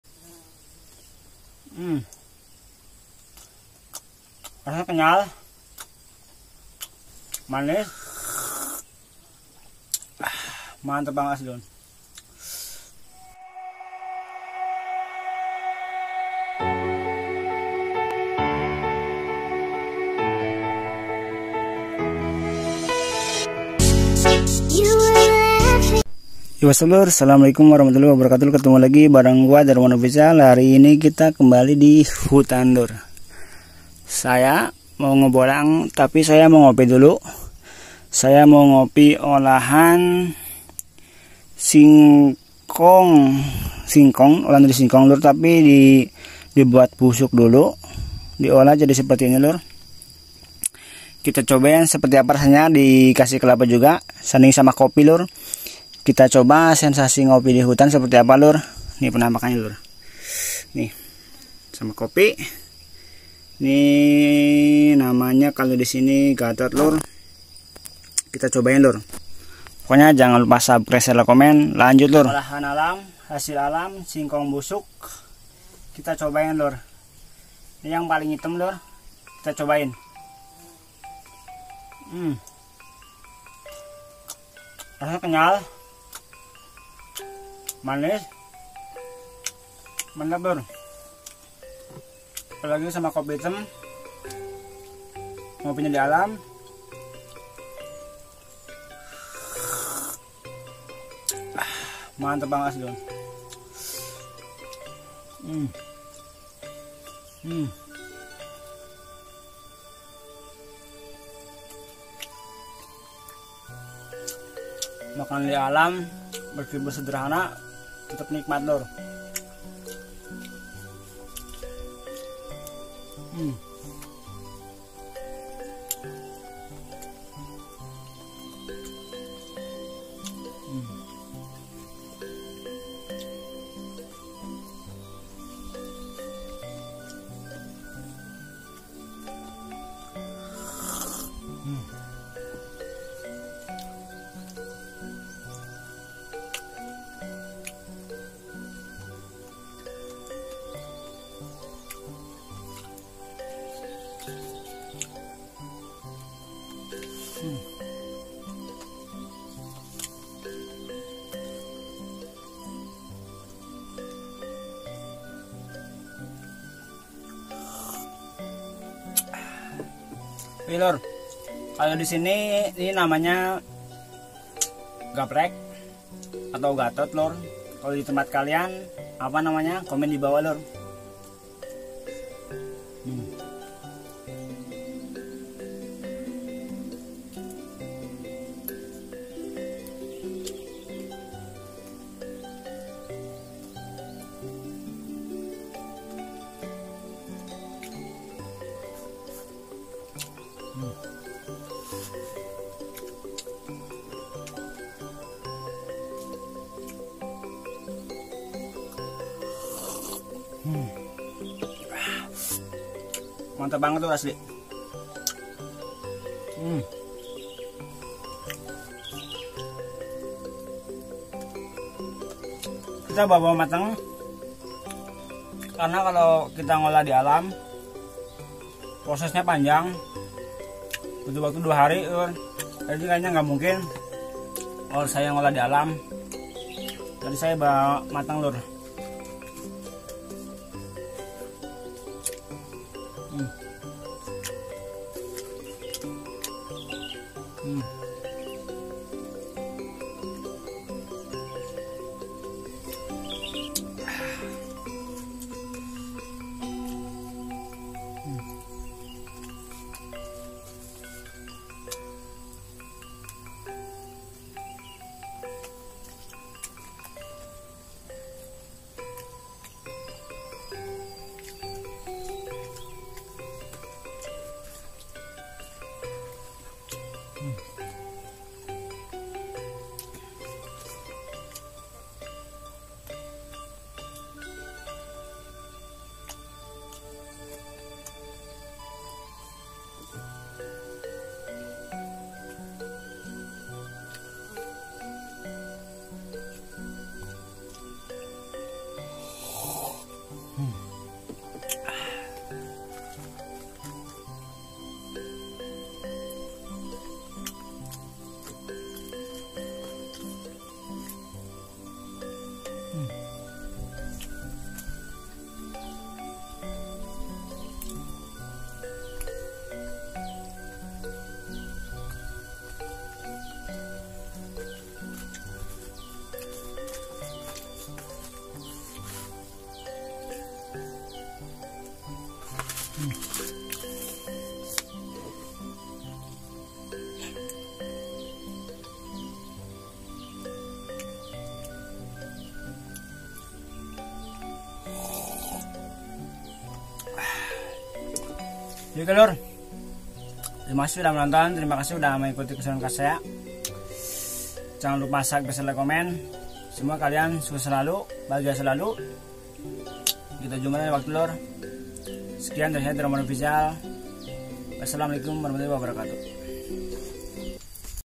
olahan singkong busuk yang kenyal dan manisss
yo whatsap lurr balik lagi bareng guaa
kali ini gua mau cobain sensasi ngopii di alam
plesss olahan singkong busuk yang kenyalll bangett